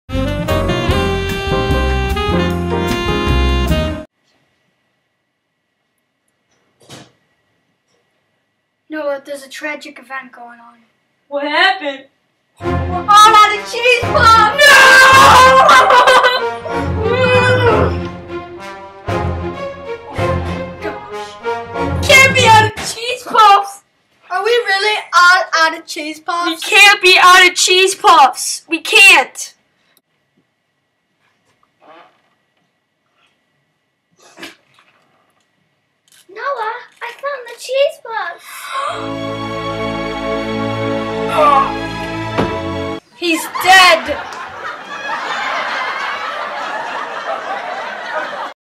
Story of The CHEESE PUFFS.
A short little movie i made with friends of mine about cheese puffs.